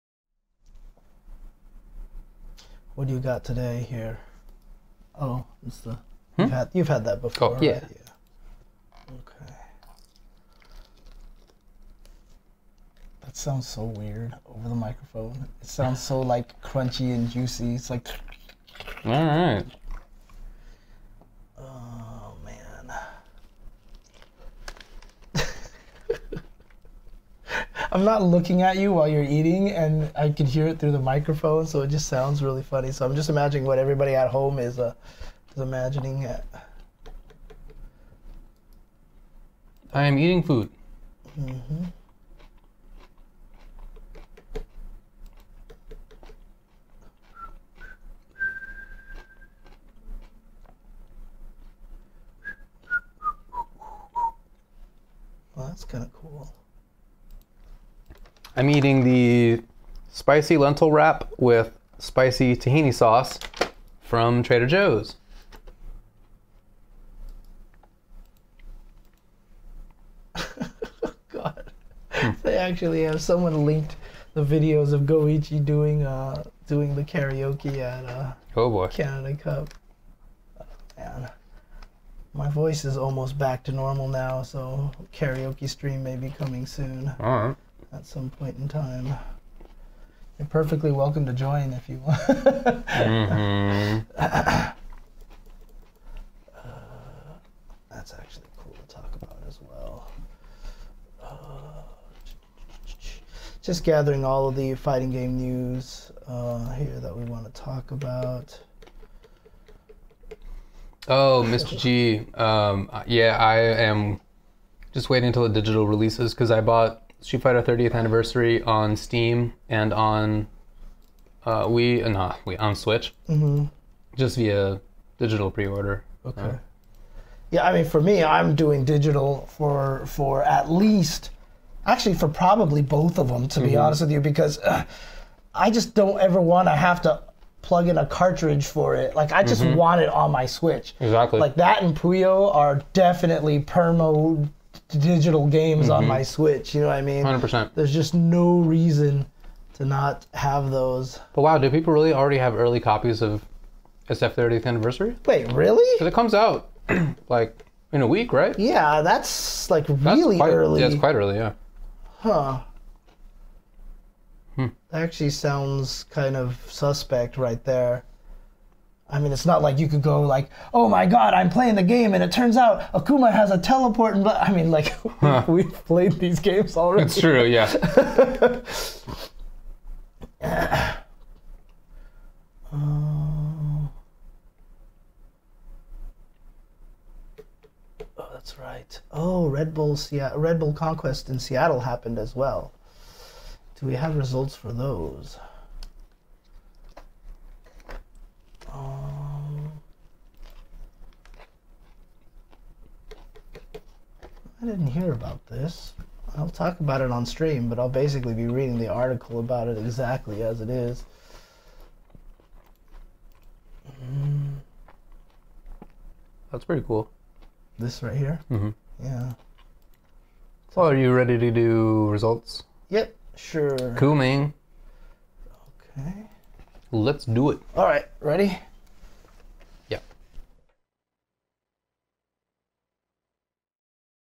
Tuesday 7.16.X: Food, Prep, and SF30A Discussion (2018-05-22)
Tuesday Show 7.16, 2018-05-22
7.16.1: Fornite $100 Million, Middle East Esports
7.16.2: Stunfest, Toryuken, Red Bull Conquest Seattle Tourney and Results
7.16.3: Combo Breaker Preview—Games, Entrants, Events
7.16.4: FGC News: DOA5 Tourney at Evo, SC6 & DBFZ Characters, Mew2King's Book
7.16.4: Chat: CSGO and Derogatory Terms, Joysticks and Theft, etc.
7.16.X: Food, Prep, and SF30A Discussion
7.16.X2: Hot Mics During Breaks, Random Chat